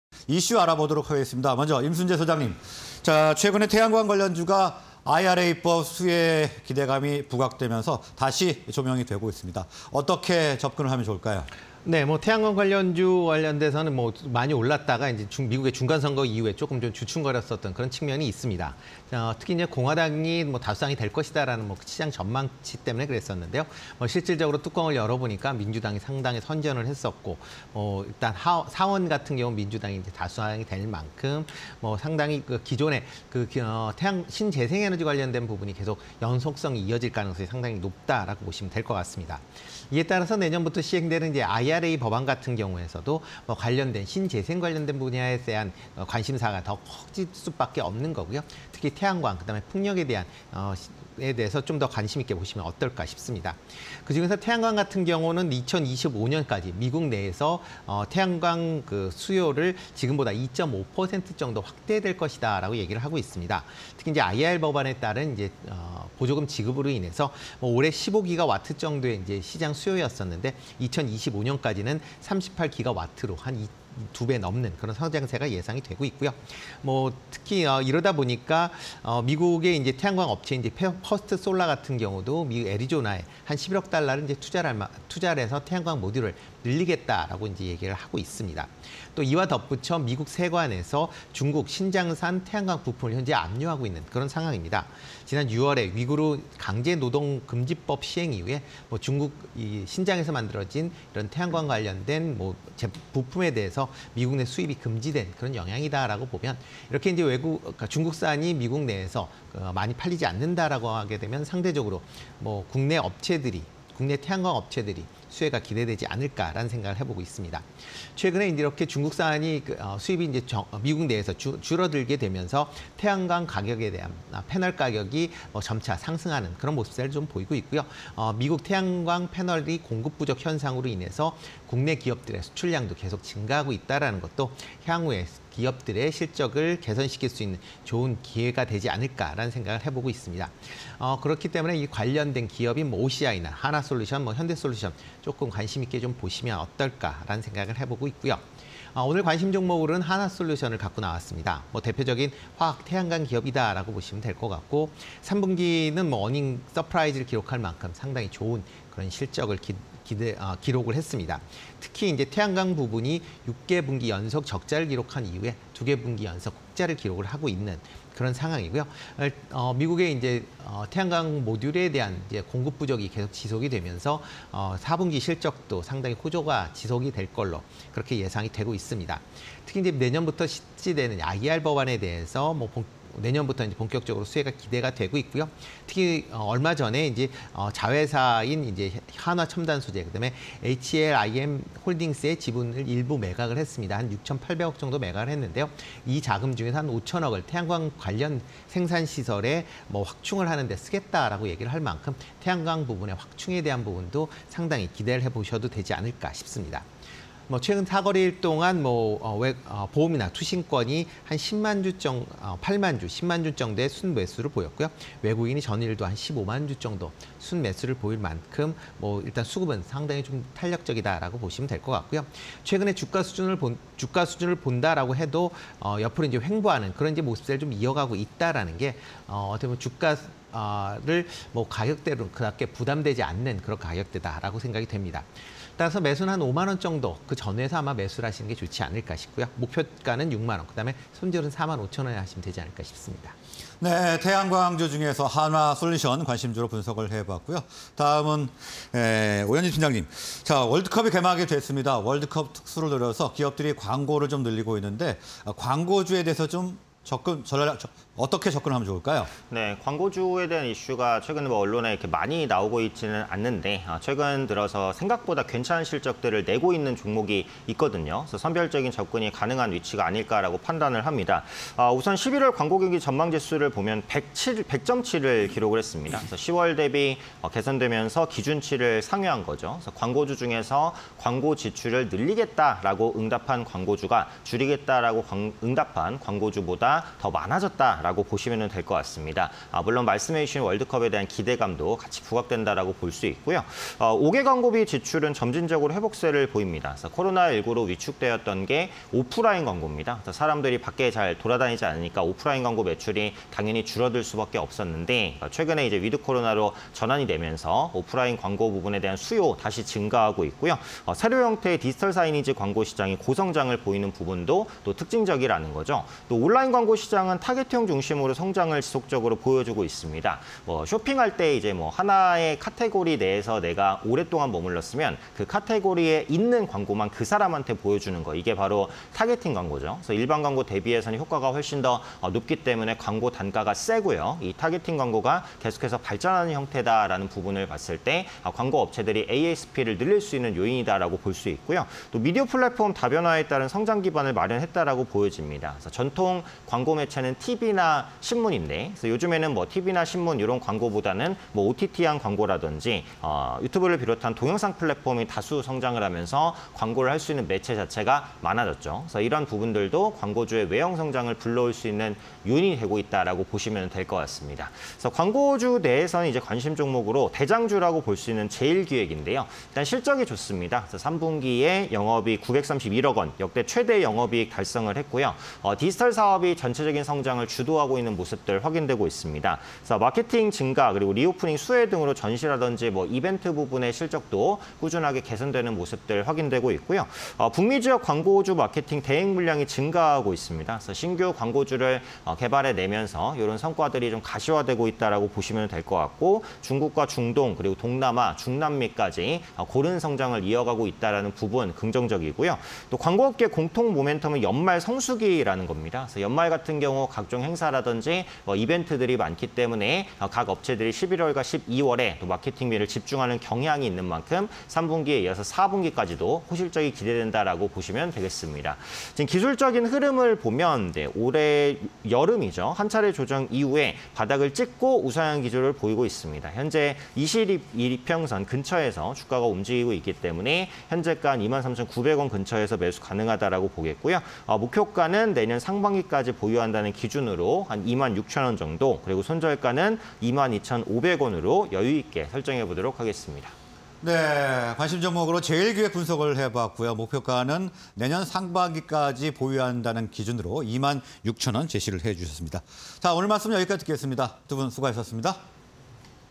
한화솔루션(009830)... 美 생산시설 및 라인 확대 속도… 태양광 기반 안정 수익 / 제일기획(030000)... 3분기 역대 최대 영업이익 달성_개장 관심주(20221122
Q. 태양광 관련주, 美 IRA 수혜 기대… 주가 볕들까?
Q. 광고주, 월드컵 특수 노린다… 상승 모멘텀 될까?

한화솔루션(009830)
- 美 생산시설 및 라인 확대 속도… 태양광 기반 안정 수익
- 3분기, 사상 최대 영업익 달성… 태양광 모듈 효과
- 4분기 신재생에너지 부문 실적 호조 지속 전망
- 내년부터 IRA 법안 관련 본격 수혜 기대

제일기획(030000)
- 3분기 역대 최대 영업이익 달성
- 북미 지역 광고주 마케팅 대행물량 증가
- 연말 성수기 시즌 돌입